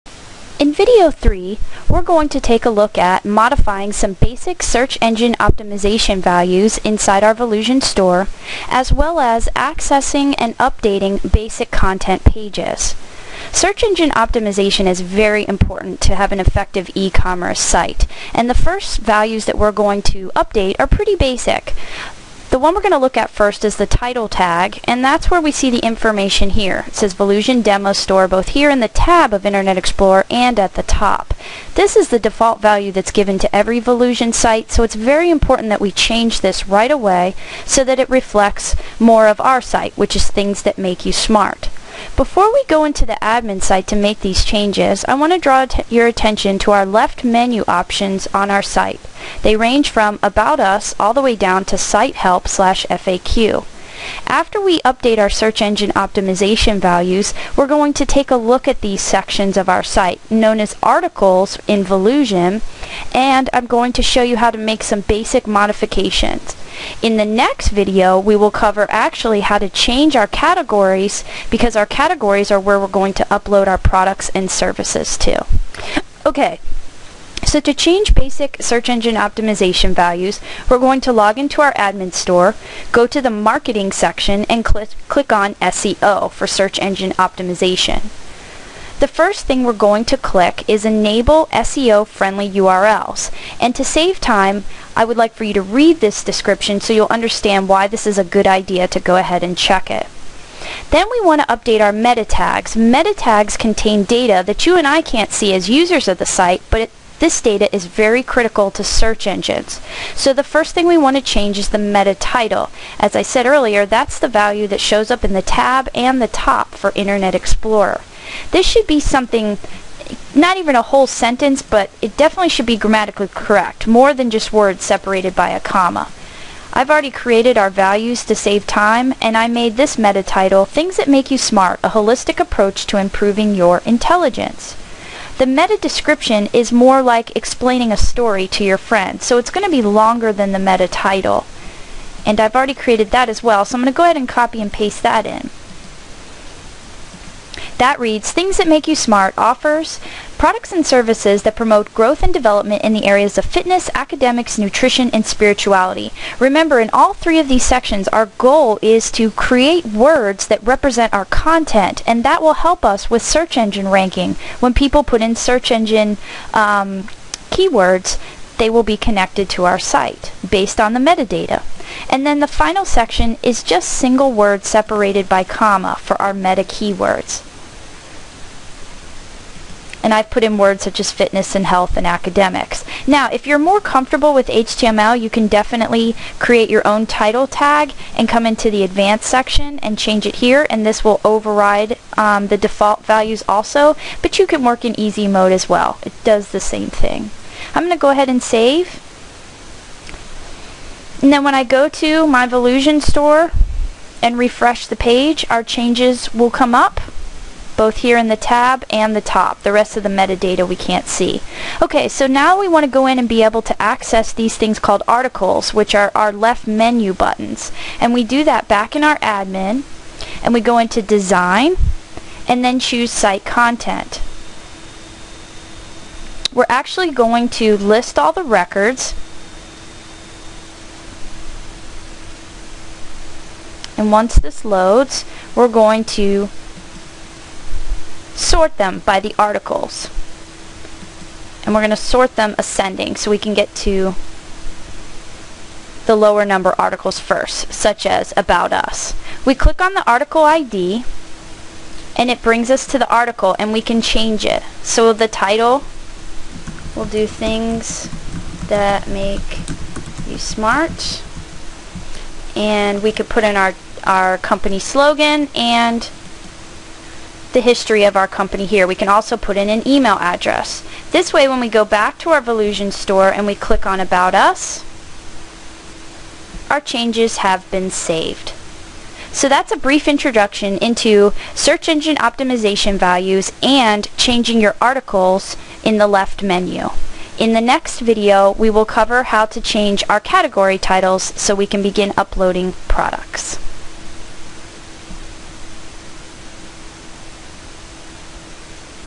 Using the Volusion Platform - Video 3
How to create an e-commerce site using the Volusion platform - Video 3

This video will address the following:
Creating content pages specific to your business (such as About Us and Shipping)
Change the default title